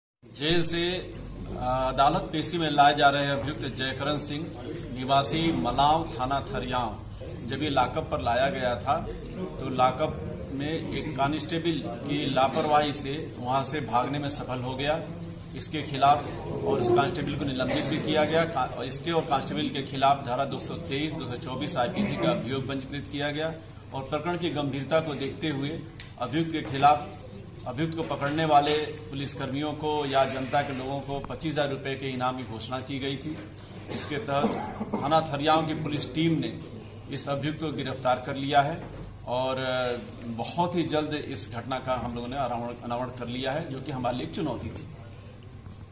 फतेहपुर: मुठभेड़ के बाद लॉकअप से फरार इनामी कैदी को पुलिस ने दबोचा
फतेहपुर: पुलिस हिरासत से बीते दिनों फरार होने वाले 25 हजार के इनामी कैदी को मंगलवार को गिरफ्तार कर लिया गया है। न्यायालय में पेशी के दौरान वो चकमा देकर भागने में सफल रहा।